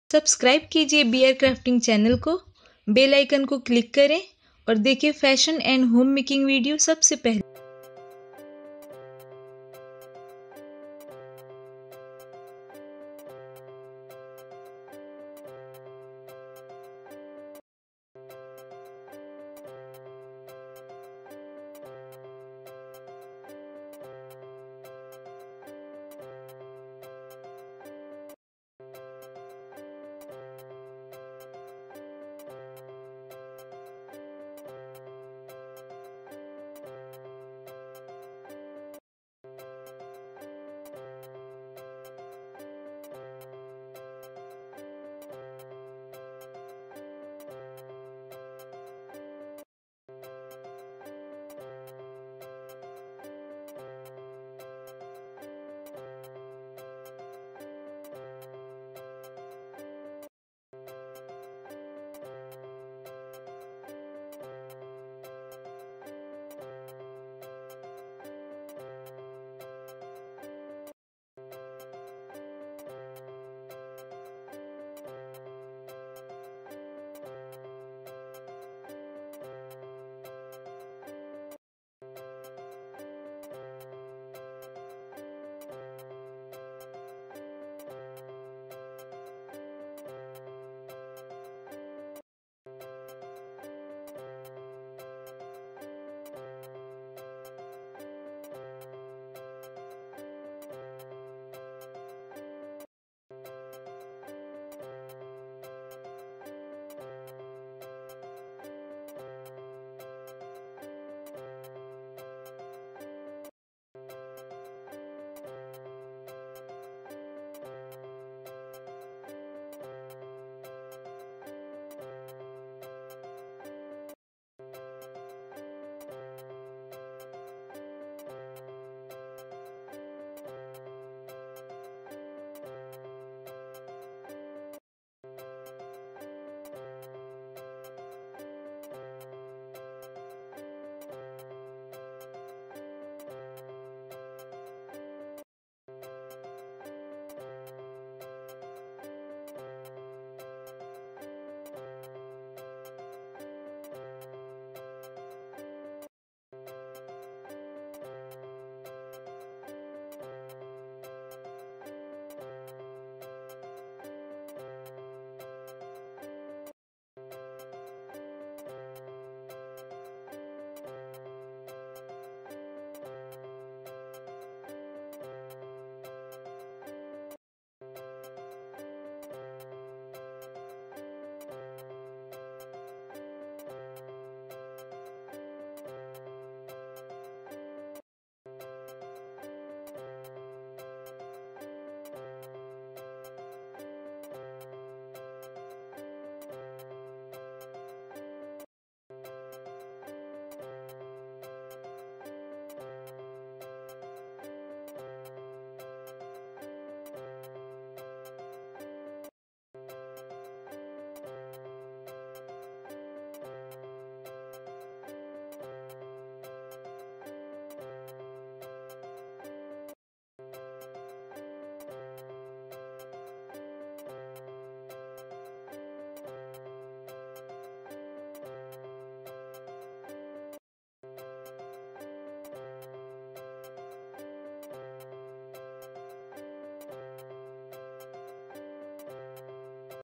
how to make home slipper with waste febric,कार्ड बोर्ड और पुराने कपडे से बनाये घर पे पहनने के चप्पल
Hello friend welcome back  today i will show you a uniqe DIY of dres and home making video ,aaj ka video bhi winter special video hai jisme ham waste chizo se slipper Banana sikhenge,hope ye video apko pasand ayega like share subscribe jarur kre thnks for watching